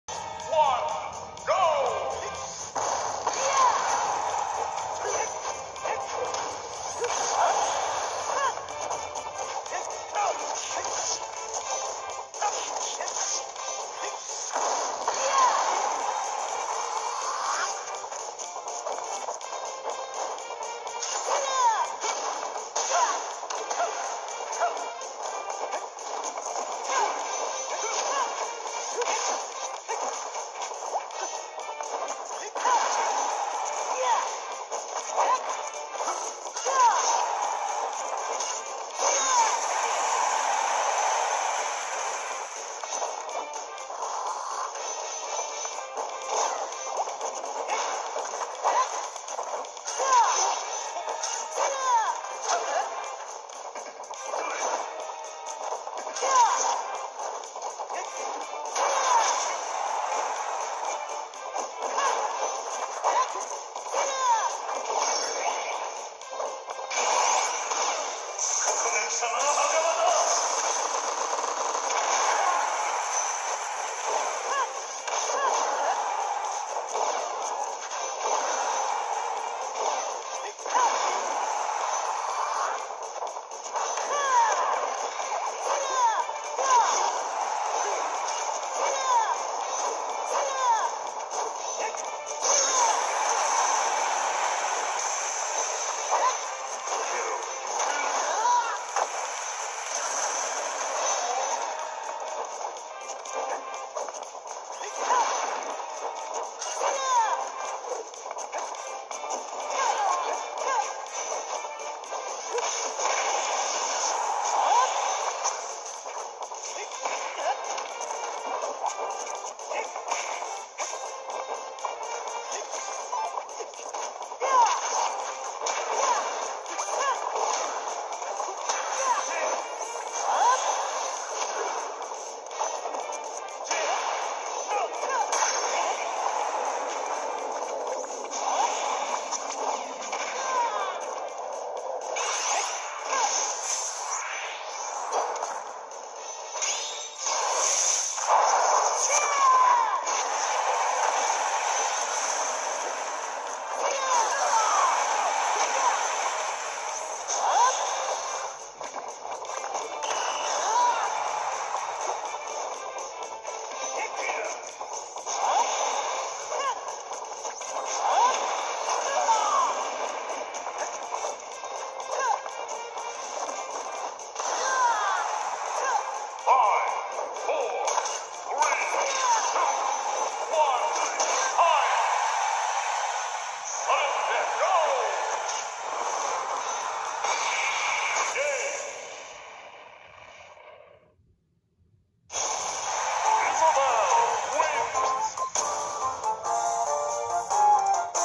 Hostile Darkness
Update - I was actually impersonating aaron and attacking people in playground arenas that day.

Kazuya was released and however, I get very mad, became hostile and started attacking everyone I am playing Hero and yes I let Tony (Isabelle) kill me in sudden death I am friends with him. My Hero is insane. I was using an alt account.

The match takes place on June 30th 2021.

This match was in Just Me's Arena, I am Hero, Tony is Isabelle, Goten is Kazuya and Blarfox is Sheik.